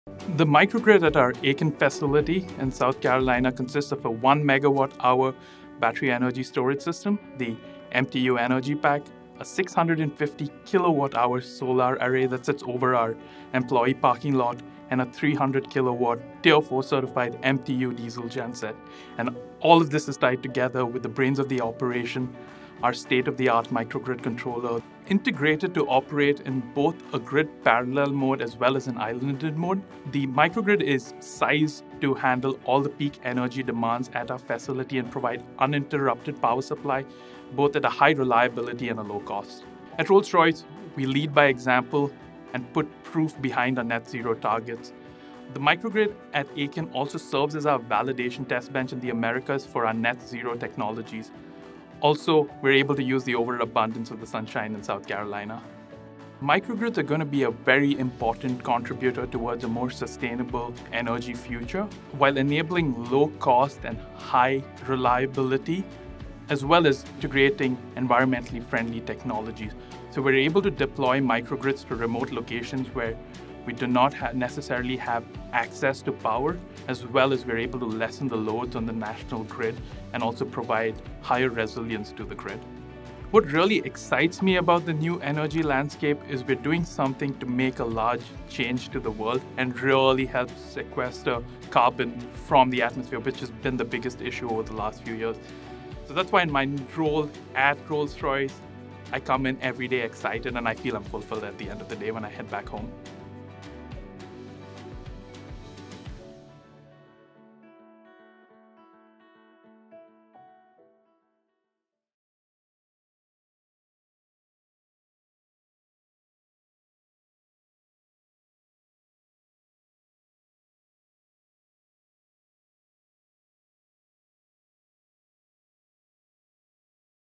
Discover the elements of the microgrid at our mtu Aiken Plant
Speaking about the recently completed microgrid at the mtu Aiken Plant in South Carolina, Sr. Manager of R&D Sustainable Solutions for the Americas, Vikram Jayanath, explains the components of the microgrid and how it is able to use the overabundance of sunshine in South Carolina to provide green energy.